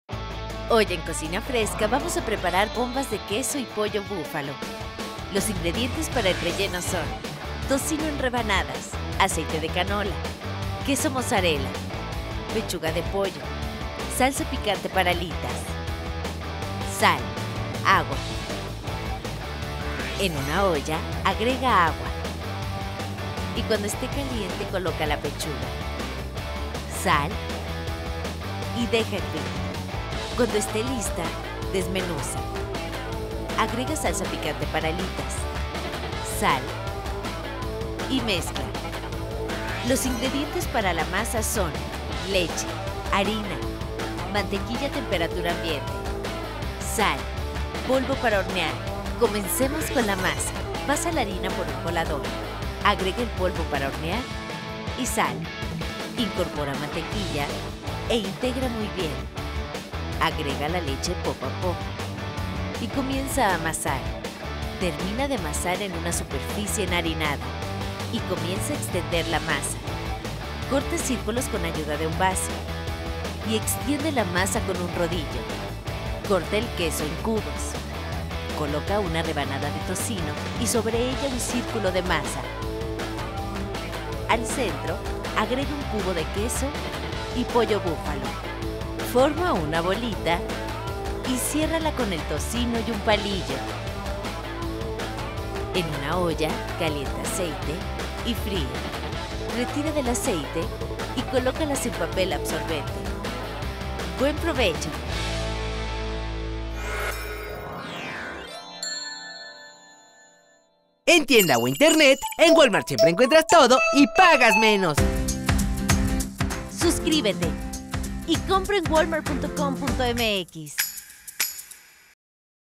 ¿Cómo preparar Bombas de Queso y Pollo Búfalo? - Cocina Fresca
Ingredientes para el relleno:
4 pzas. de tocino en rebanadas
2 tzas. de aceite de canola
400 gr. de queso mozzarella
1 pza. de pechuga de pollo
1/2 tza. de salsa picante para alitas
1 cda. de sal
1 lt. de agua

Ingredientes para la masa:
3/4 tza. de leche
2 tzas. de harina
1/2 barra de mantequilla a temperatura ambiente
1 cdta. de sal
1 cdta. de polvo para hornear

Procedimiento:
En una hoya agrega agua y cuando este caliente coloca la pechuga, sal y deja hervir, cuando este lista; desmenuza, agrega salsa picante para alitas, sal y mezcla. Comencemos con la masa, pasa la harina por un colador, agrega el polvo para hornear, sal, incorpora mantequilla e integra muy bien. Agrega la leche poco a poco y comienza a amasar, termina de amasar en una superficie enharinada y comienza a extender la masa. Corta círculos con ayuda de un vaso y extiende la masa con un rodillo, corta el queso en cubos, coloca una rebanada de tocino y sobre ella un círculo de masa, al centro agrega un cubo de queso y pollo búfalo. Forma una bolita y ciérrala con el tocino y un palillo. En una hoya calienta aceite y fríe, retira del aceite y colócalas en un papel absorbente.

Todo lo necesario para hacer esta receta lo puedes encontrar en Walmart.